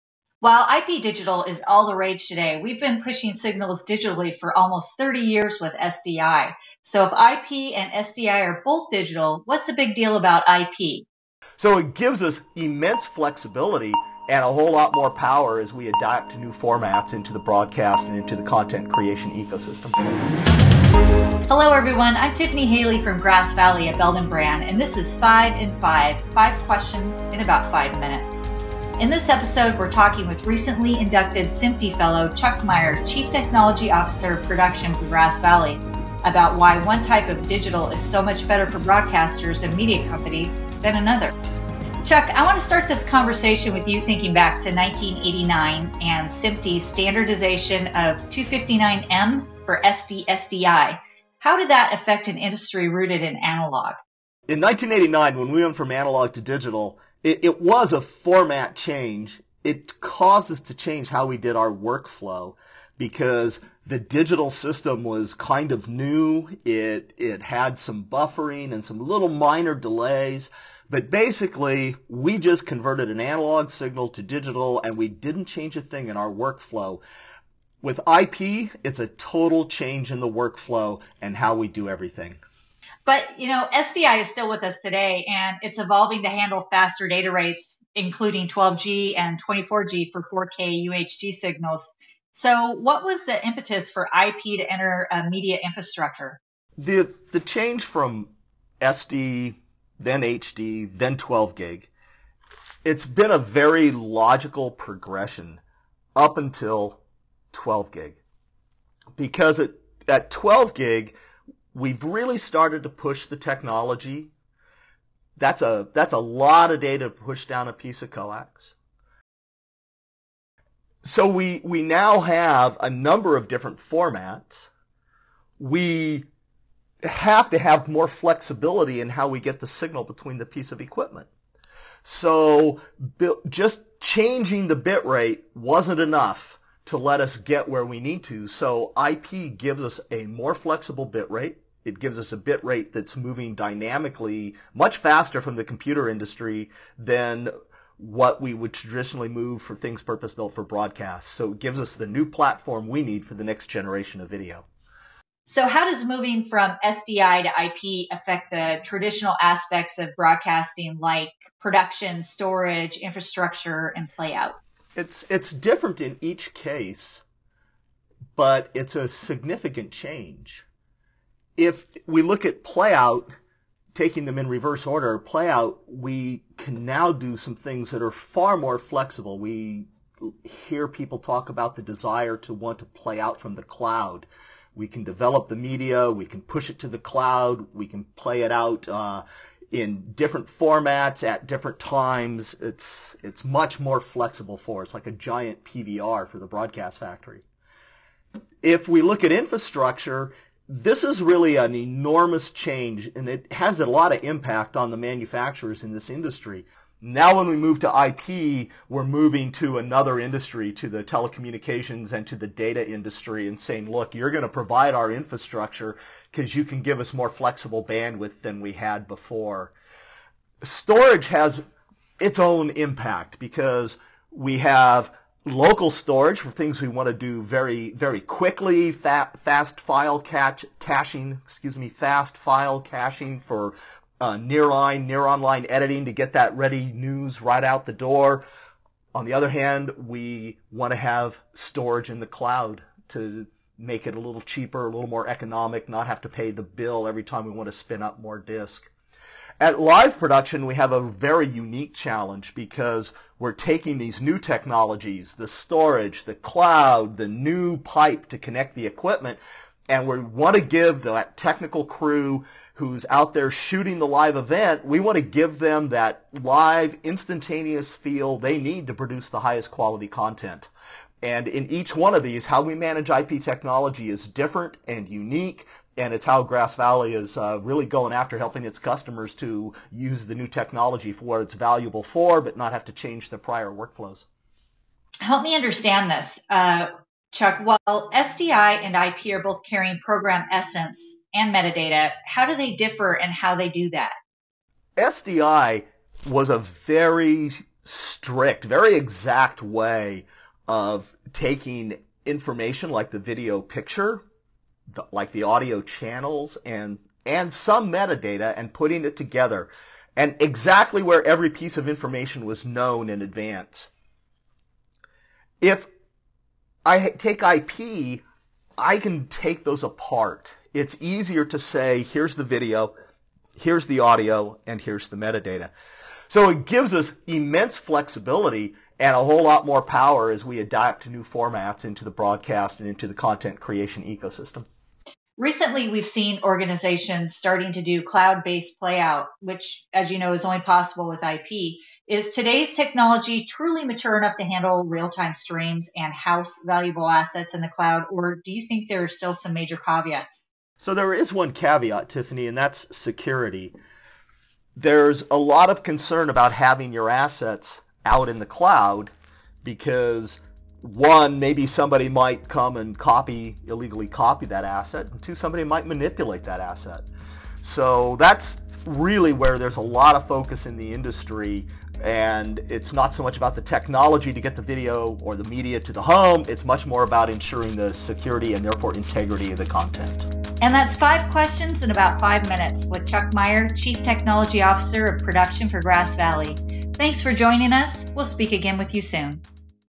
A Question of Digital: SDI vs IP - Five in 5 Podcast
While IP digital is all the rage today, we’ve been pushing signals digitally for almost 30 years with SDI. So if IP and SDI are both digital, what’s the big deal about IP?
In this episode, we're going to talk with Chuck Meyer, Chief Technology Officer, Production for Grass Valley and recently inducted SMPTE Fellow, about why one type of digital is so much better for broadcasters and media companies than another.